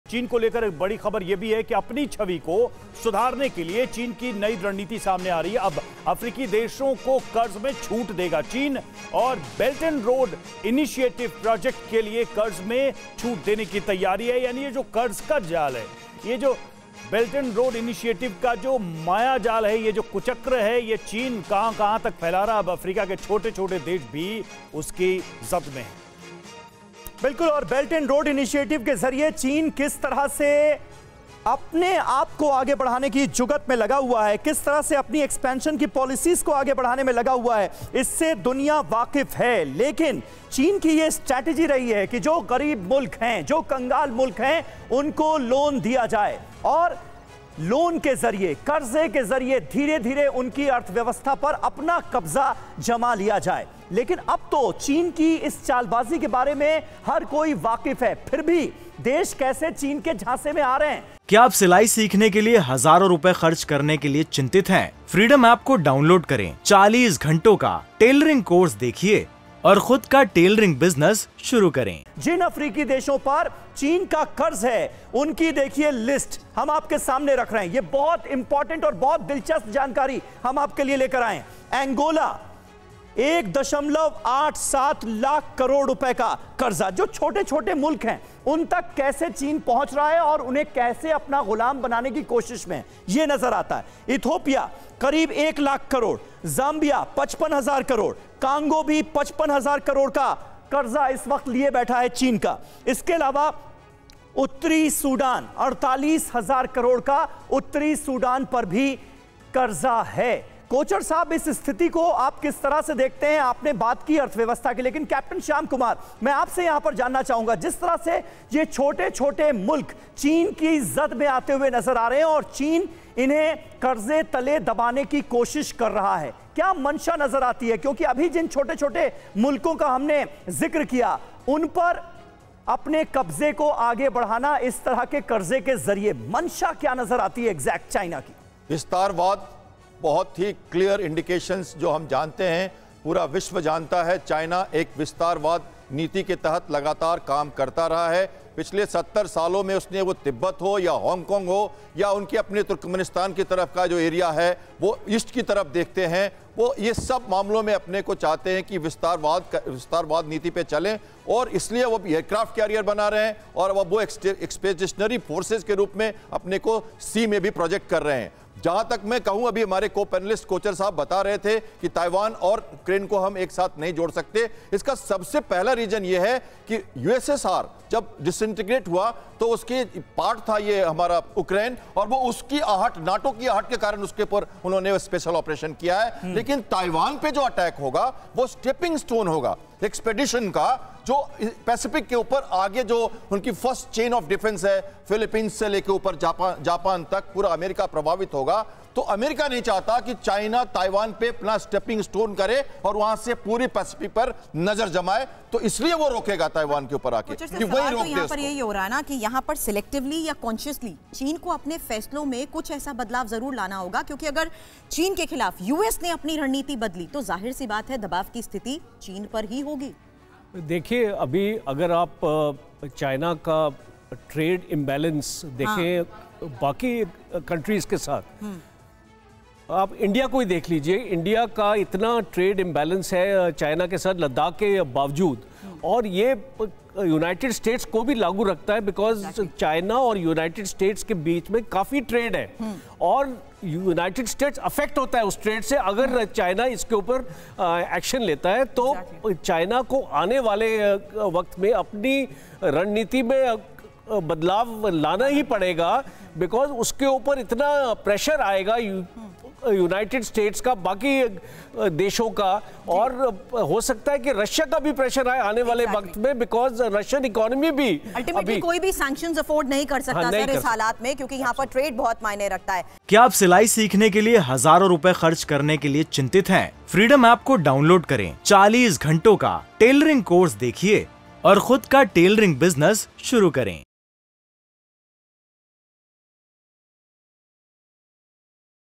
China Taiwan War: अब अफ्रीकी देशों को चीन कर्ज में छूट देगा चीन | Xi Jinping
चीन और ताइवान (China Taiwan War) के बीच विवाद बढ़ता ही जा रहा है. इसी बीच अपनी छवि सुधारने के लिए चीन ने नई रणनीति बनाई है. अब अफ्रीकी देशों को चीन कर्ज में छूट देगा..बेल्ट एंड रोड इनिशिएटिव प्रोजेक्ट के लिए कर्ज में छूट देने की तैयारी.
Credit